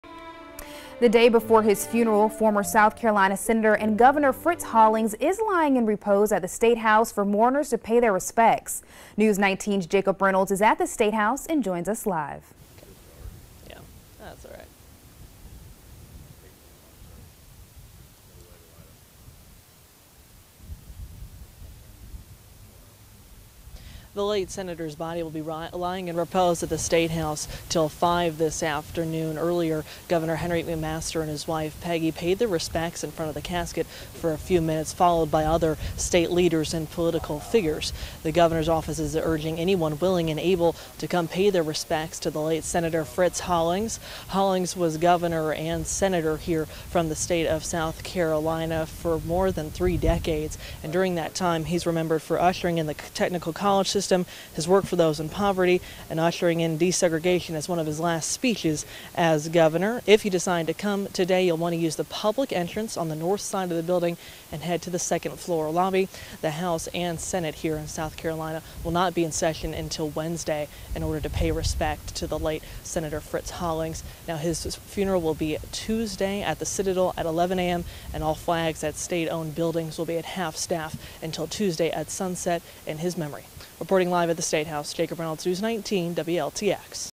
Fritz Hollings lies in State at SC Capitol
Fritz Hollings, who served as a South Carolina governor and senator, is being given a special tribute inside South Carolina's capitol.